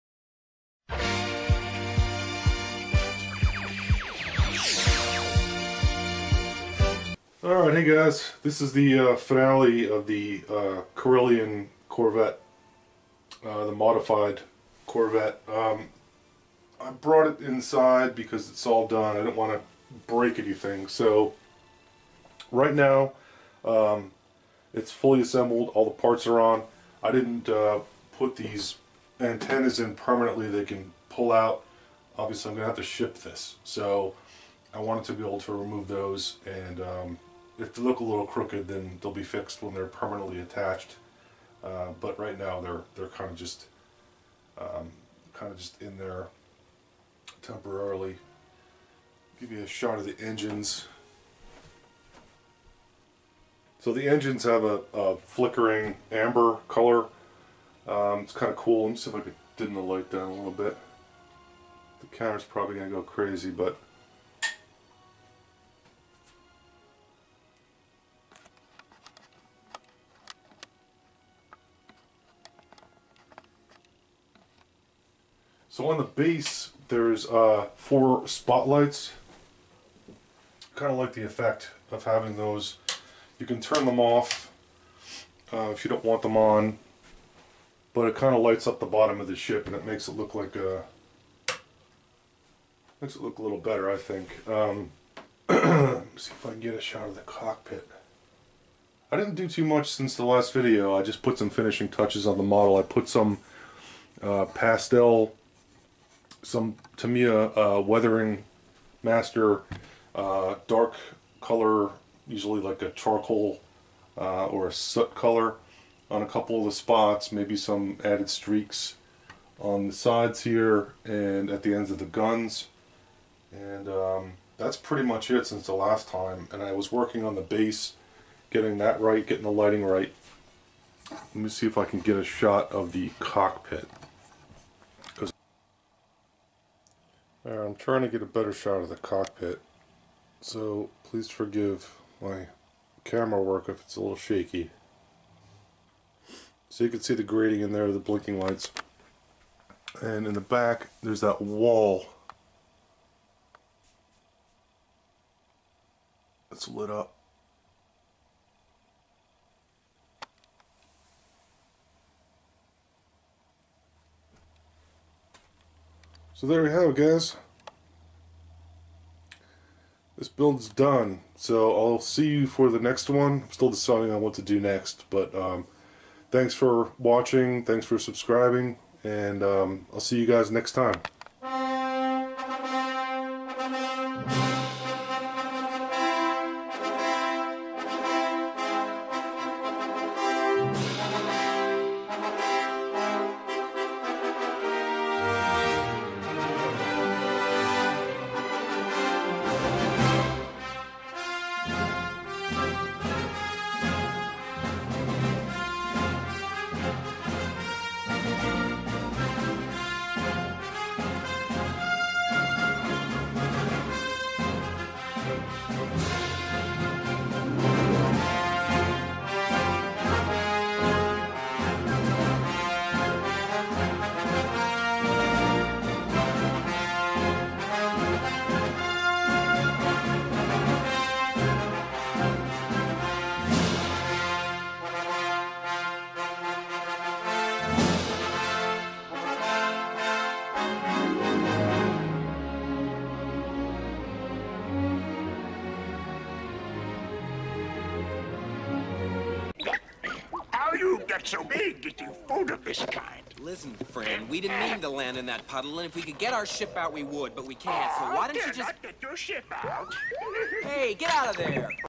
Corellian Corvette/Gunship Concept Model Build Finale
This is a build-up of a modified Randy Cooper Corellian Corvette resin kit into a concept model.